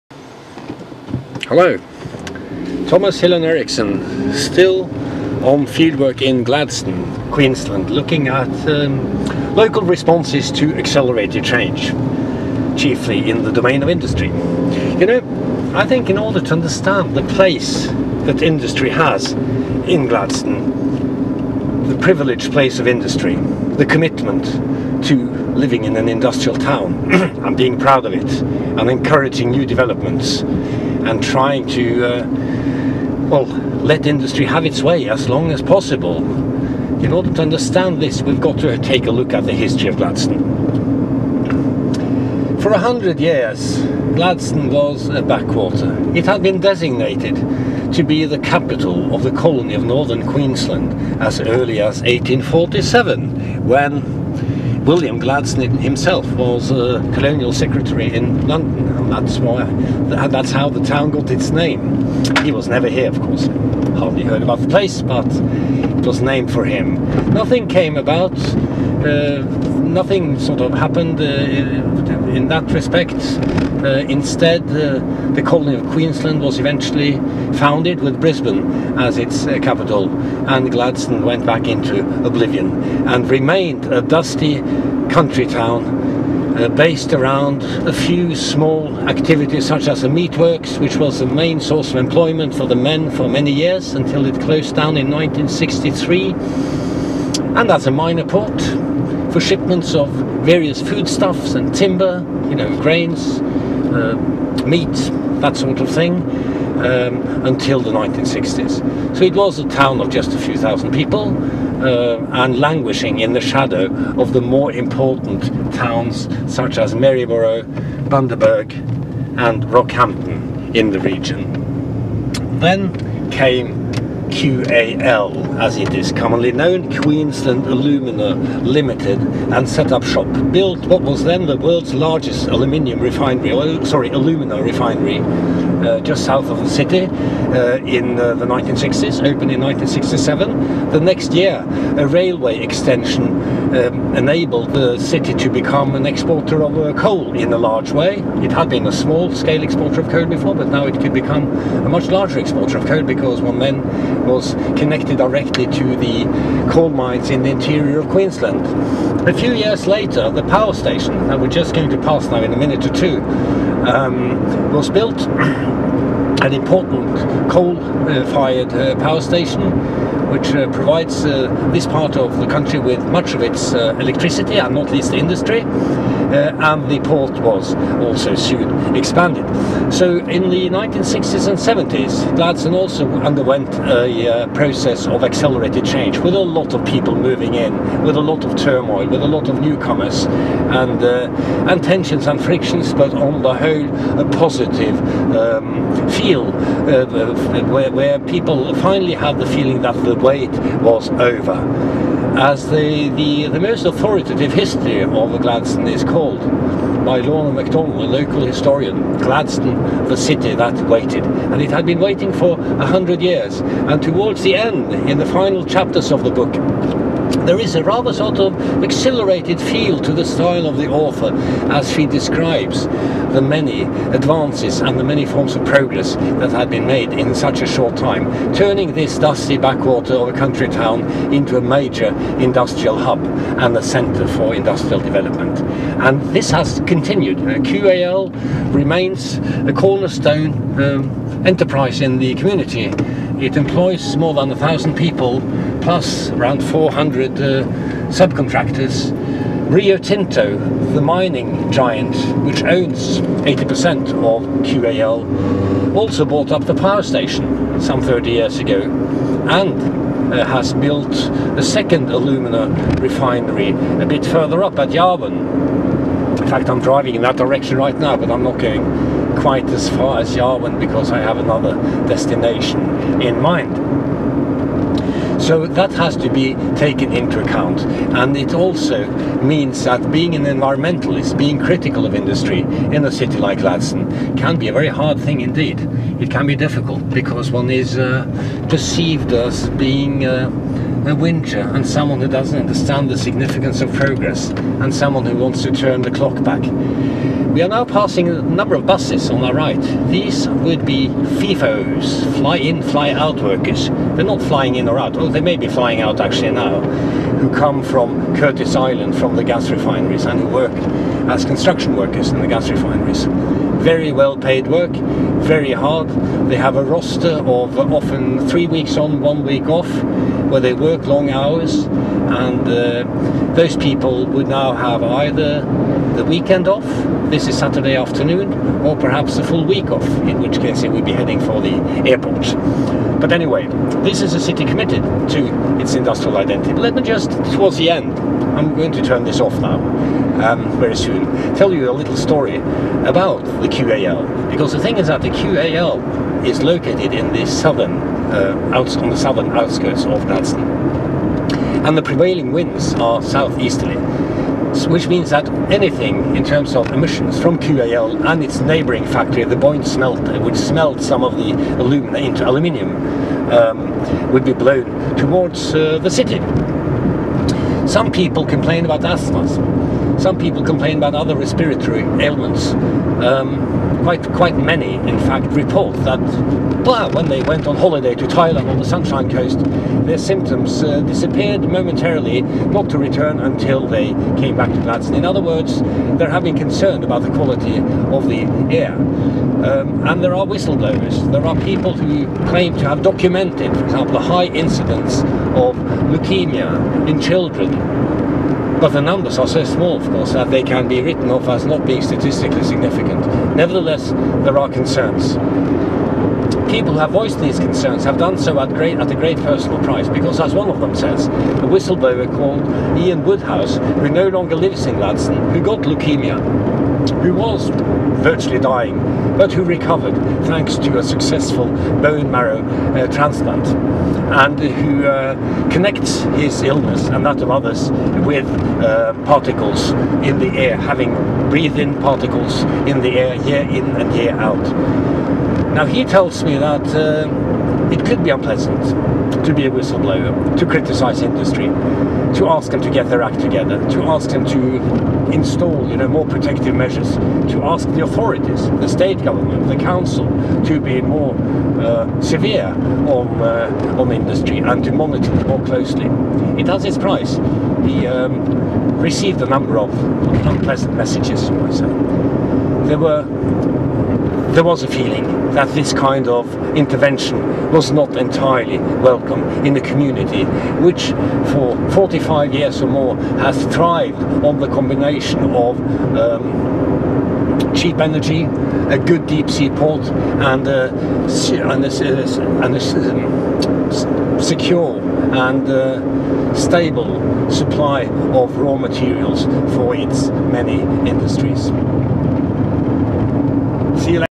Driveabout on industry
Thomas Hylland Eriksen takes you on a guided tour of his research site in Australia, part 4